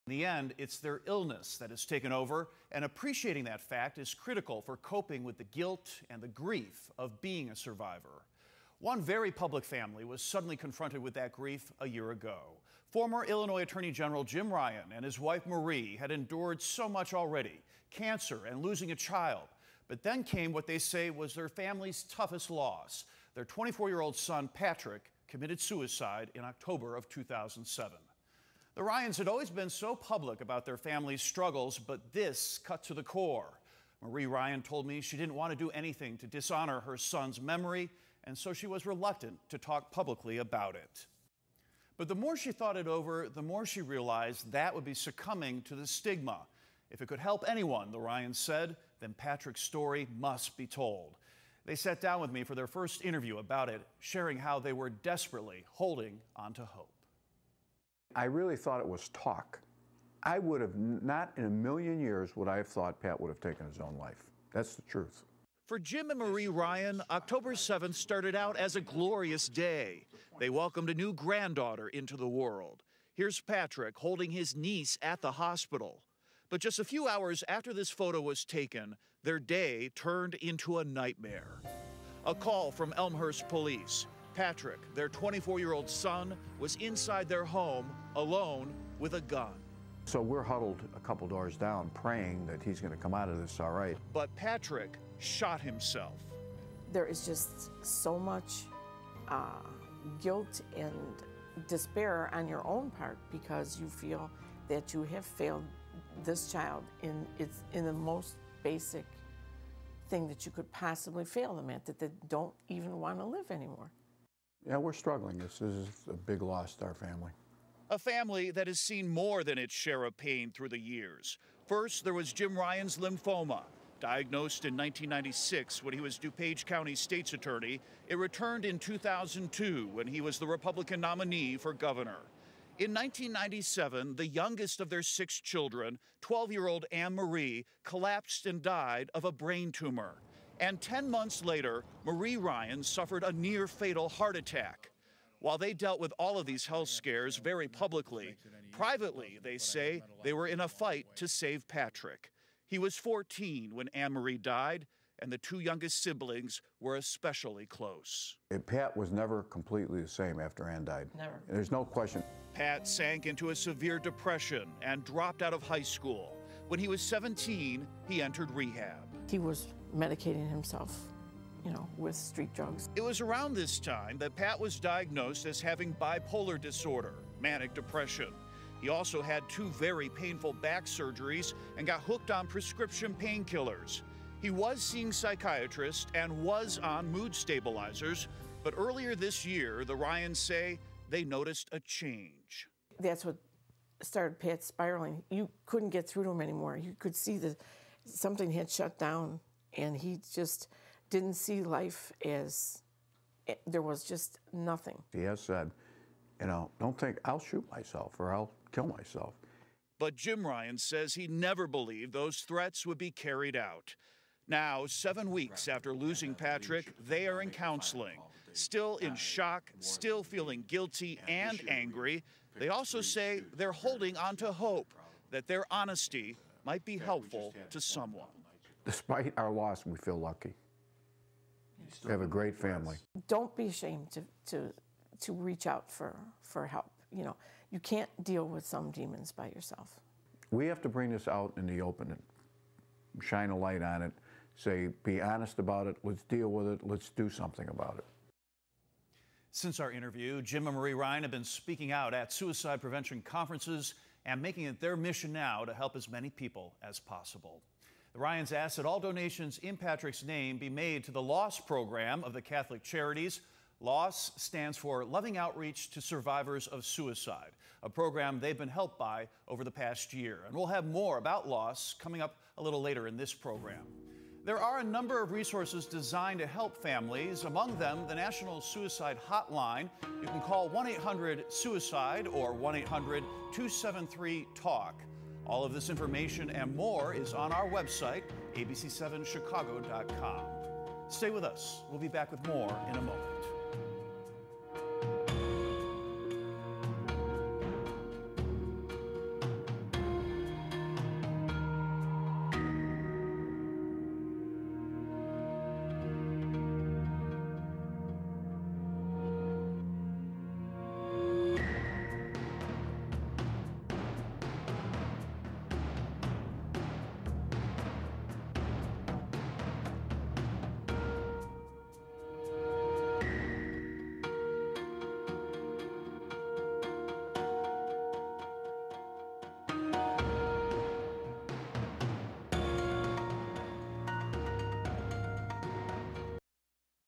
Holding On To Hope
EXCLUSIVE:  Former Illinois Attorney General and candidate for Governor, Jim Ryan (R-Elmhurst,) and his wife, Marie, granted me their first and only interview after losing their son, Patrick, to suicide.  The Ryans battled cancer and lost a young child while in the media spotlight.  They told me nothing comes close to suicide.
They credit LOSS, Loving Outreach to Survivors of Suicide, a support group run by the Catholic Charities of Chicago, for helping them cope with the unique grieving process.
Produced by Kevin Roy and Stephen Lewis, ABC7Chicago, WLS-TV, in conjunction with The Carter Center's Mental Health Journalism Program, Atlanta.